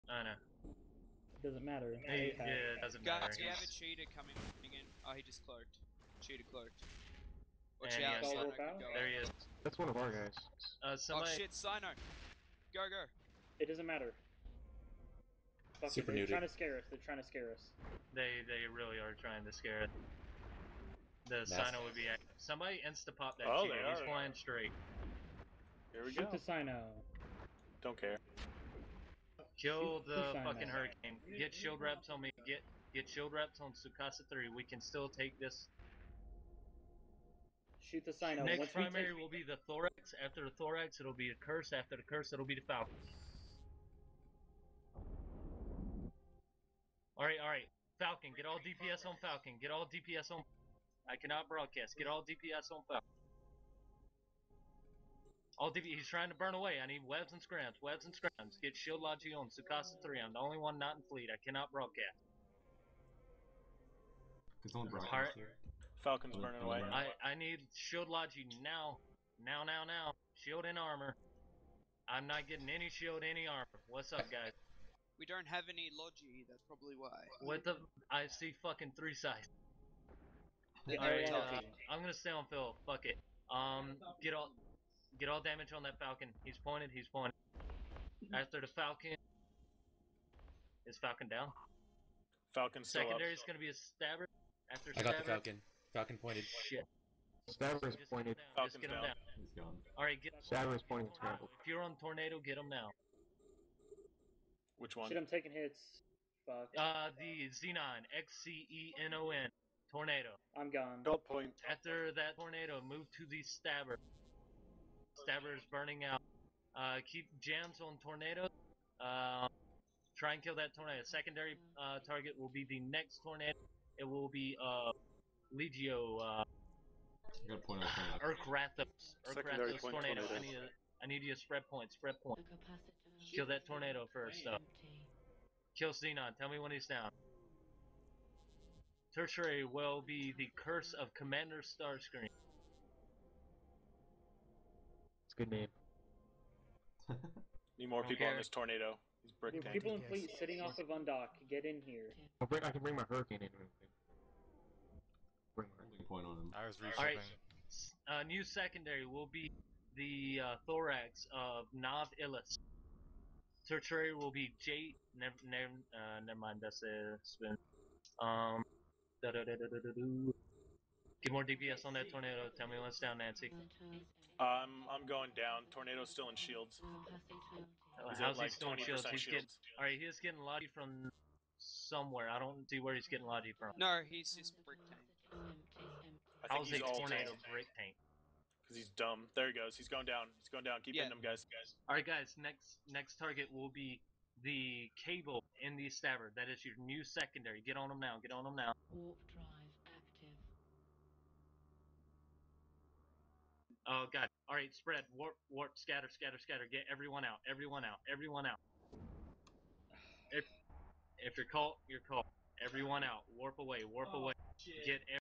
A Blackbird's Life
Arriving to a flight in progress, the brief story of a Brave Newbie Blackbird pilot and his fate.